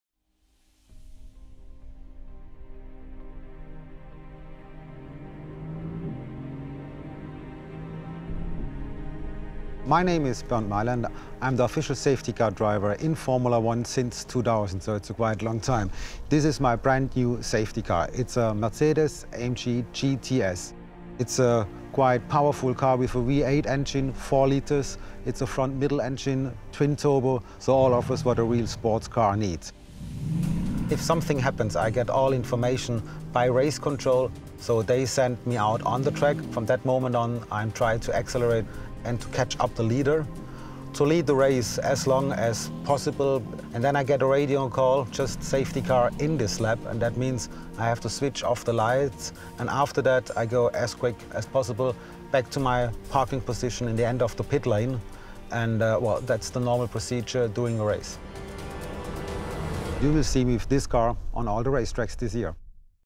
Introducing the 2015 Formula One Safety Car - The Mighty Mercedes-AMG GT S!
At the first Grand Prix of the 2015 season this weekend in Melbourne, Australia, the Mercedes-AMG GT S will make its debut as the Official Safety Car of the FIA Formula One World Championship!